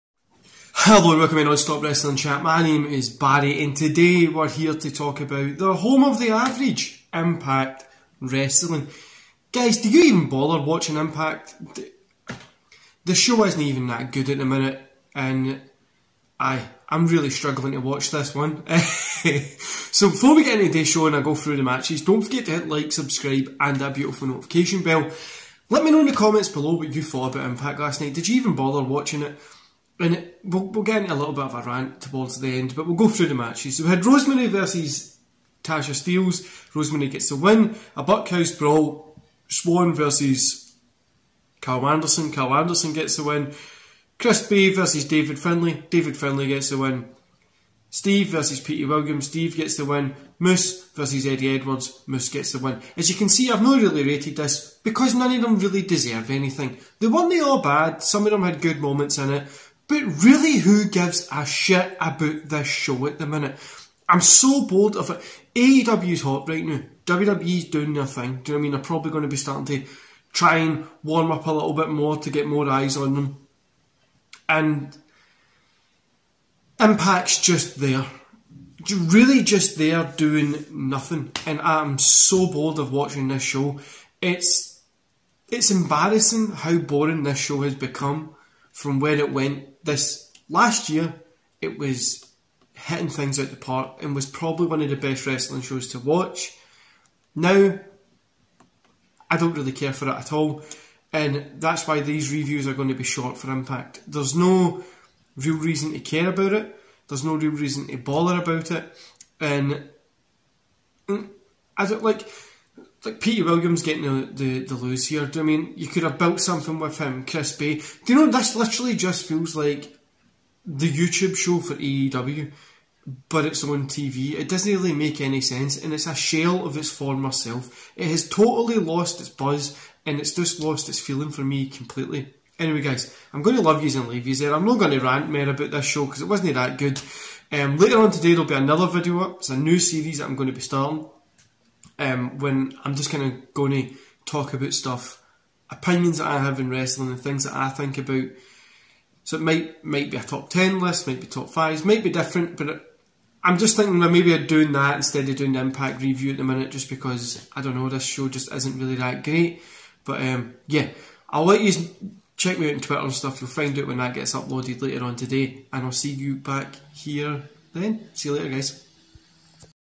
Be Average Become Impact - Impact Wrestling Review 9th September 2021
Nothing really to write home about on this one at all. If you skipped this weeks impact that's okay.

***PLEASE NOTE***
All views expressed in these videos are my own.